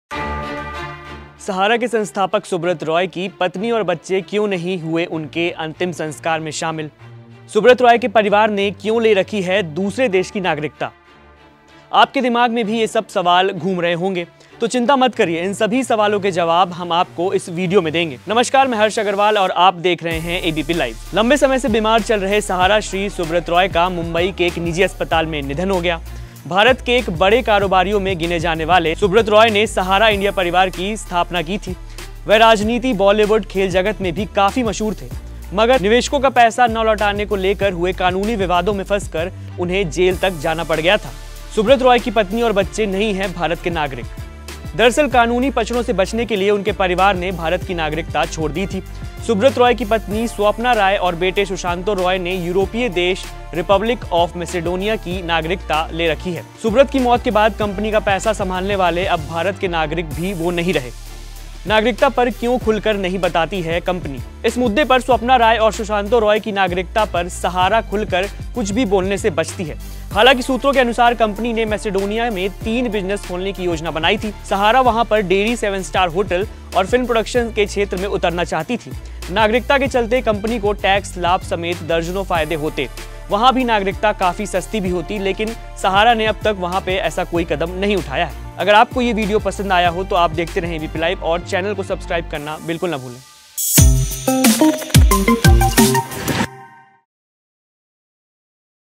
देखिए क्यों Subrata Roy के पत्नी और बेटे अंतिम संस्कार में नहीं हुए शामिल
हजारों करोड़ों संपत्ति के मालिक सुब्रत रॉय के अंतिम संस्कार में उनके अपने दोनों बेटे और पत्नी समेत कोई शामिल नहीं हुआ...